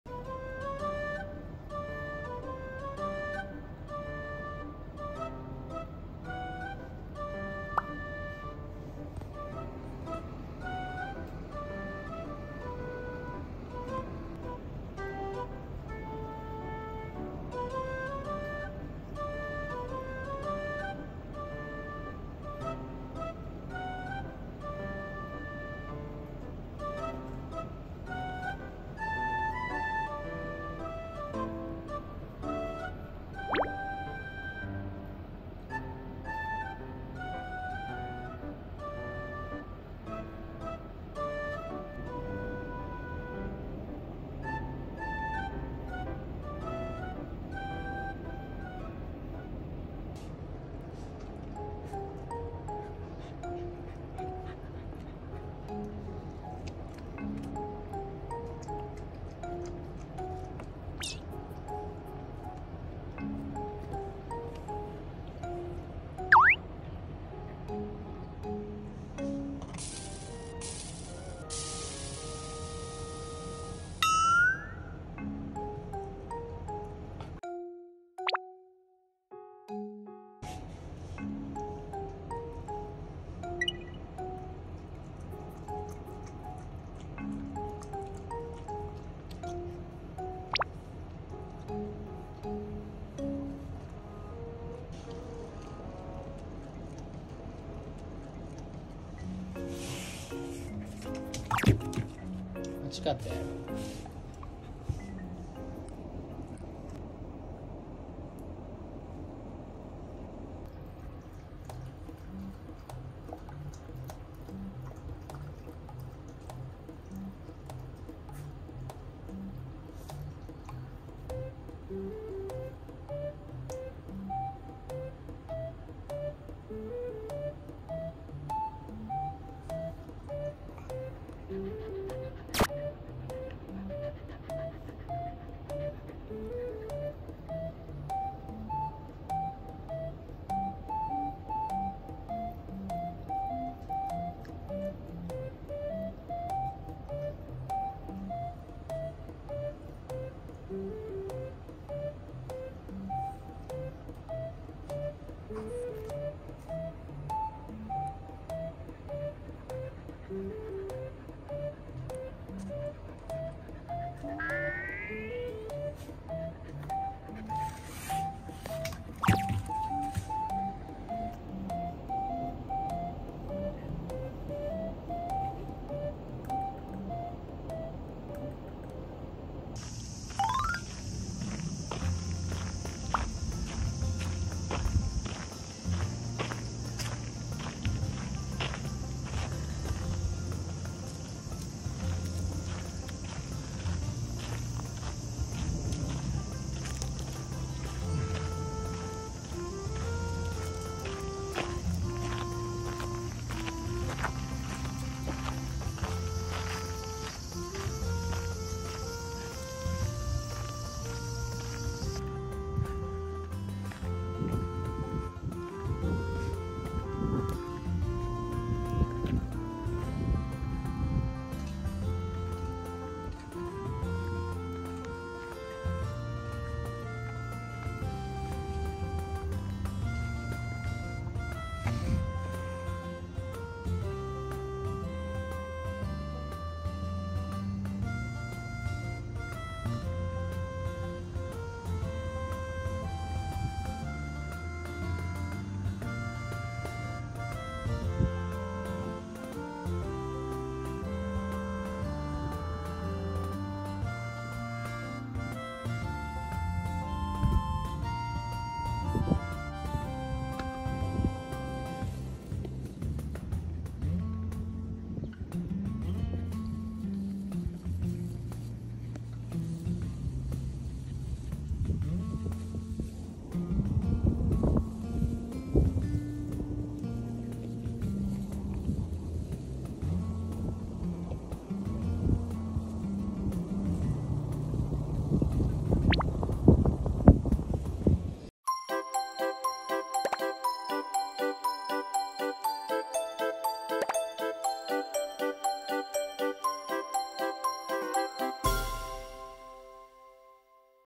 生まれてはじめての暑さに敗北した柴犬の子犬Shiba Inu puppy
お天気の良い日にベランダで日向ぼっこを楽しむおふたりさん
暑さ対策にお水も用意しましたが
そのお水を飲むためにつくねは生まれてはじめての床熱体験
テンは流石お兄ちゃんの貫禄を見せつけました